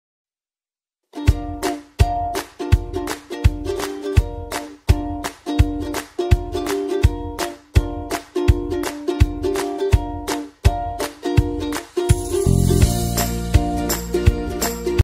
Flower bouquets delivered for all occasions 💐 | InstaShop
Forgot a birthday or anniversary and need a last minute personalized gift? 💭 Say it with flowers! 💐 Check out the Flower Shops section on InstaShop and schedule your delivery now. 😍